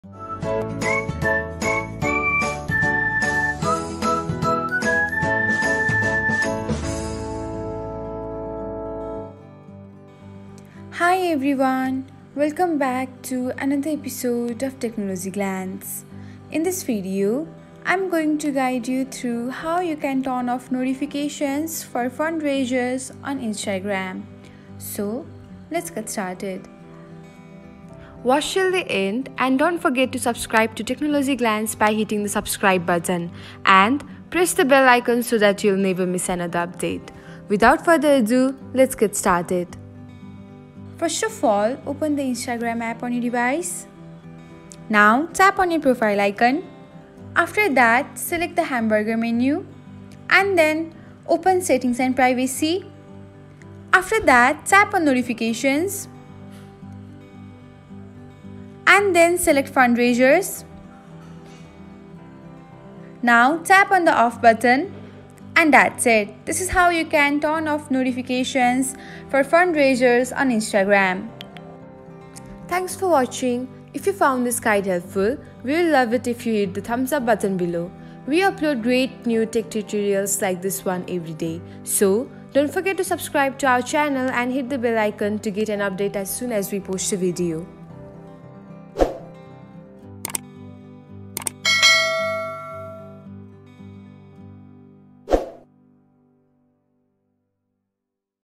How To Turn Off Notifications For Fundraiser On Instagram? | Technologyglance
In this video, we'll guide you on how to turn off notifications for fundraisers on Instagram, giving you control over the notifications you receive and allowing you to customize your Instagram experience. If you're interested in managing your notifications and reducing the frequency of fundraiser-related alerts on Instagram, this tutorial is for you.

Turning off notifications for fundraisers ensures that you receive fewer or no notifications whenever a fundraiser is launched, updated, or reaches important milestones. We'll walk you through the step-by-step process of accessing the Instagram app, navigating to the settings, and disabling notifications specifically for fundraisers. By following these simple steps, you'll be able to tailor your notification preferences to suit your needs.

Not only will you learn how to turn off notifications for fundraisers on Instagram, but we'll also provide insights into the reasons why you might choose to do so and alternative ways to support causes and stay informed about fundraising initiatives. We'll empower you to manage your Instagram experience and focus on the types of notifications that are most relevant to you.